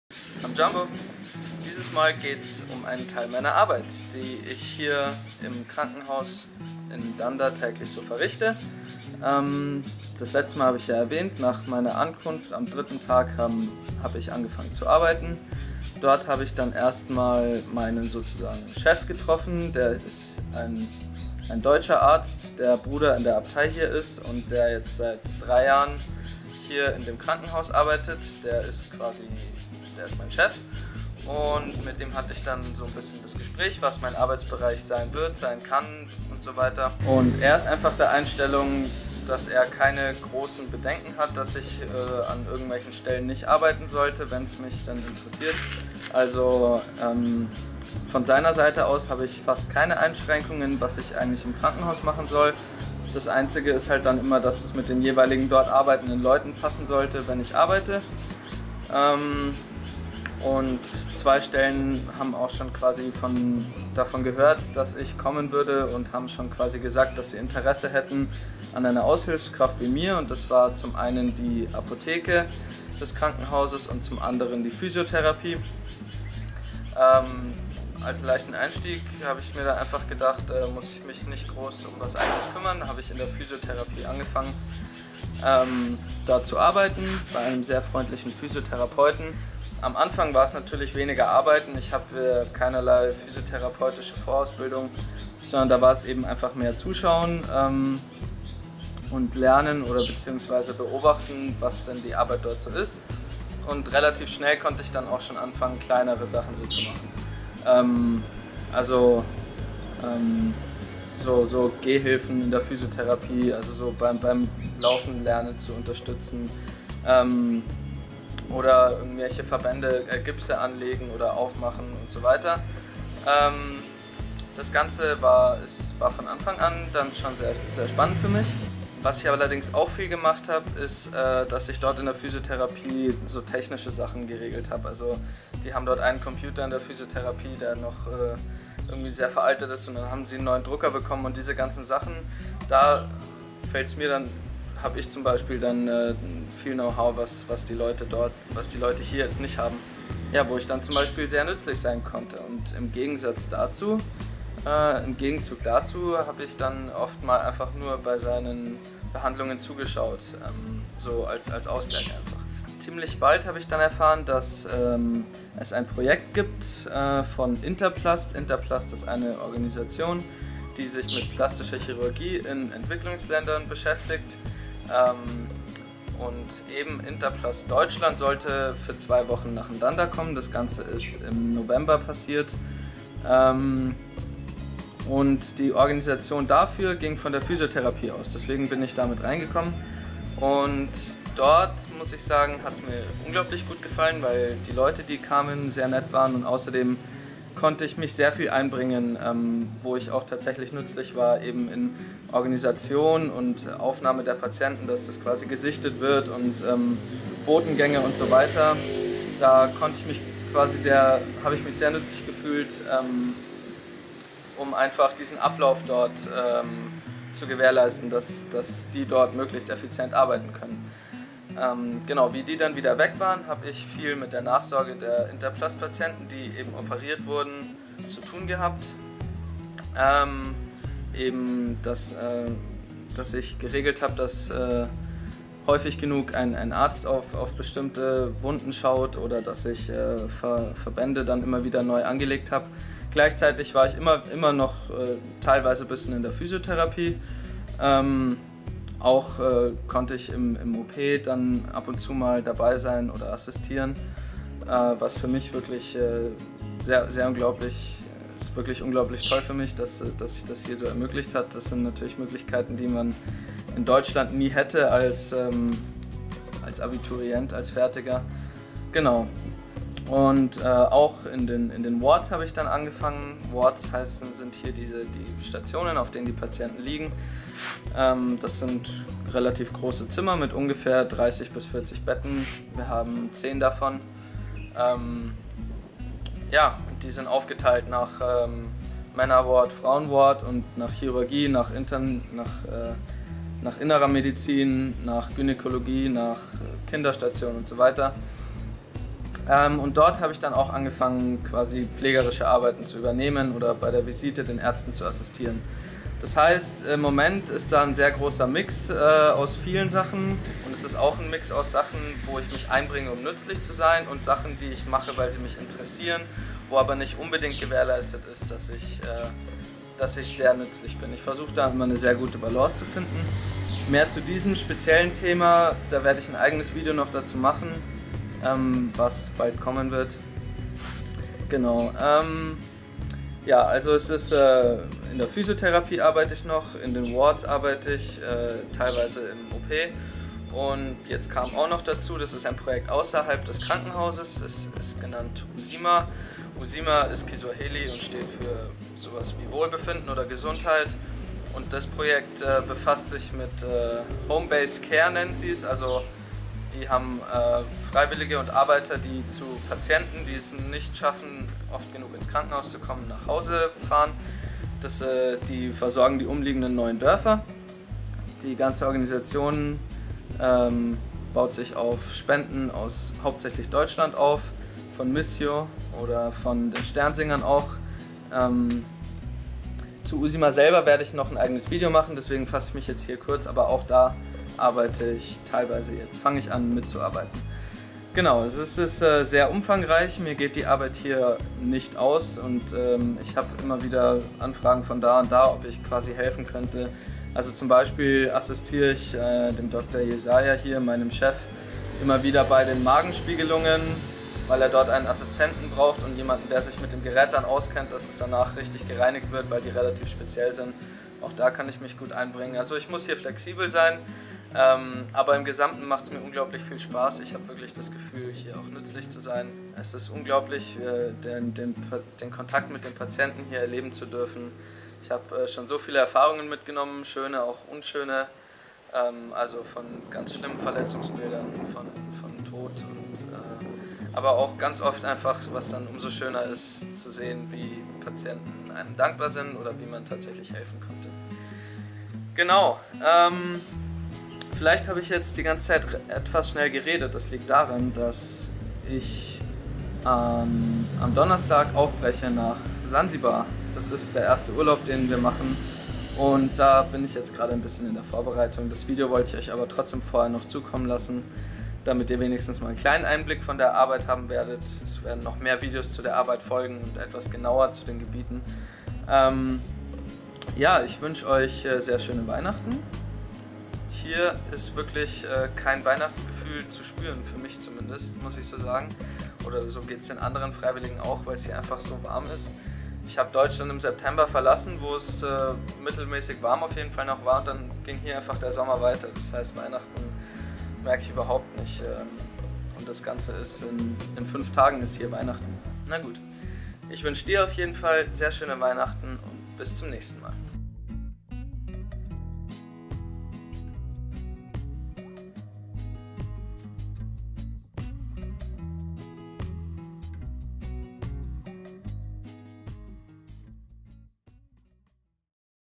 Meine Arbeit - Teil 1
Bevor es für mich in meinen ersten Urlaub geht melde ich mich nochmal mit einem groben Überblick über meine Arbeit. Genauere Videos und mehr Bildmaterial folgt dann im Januar.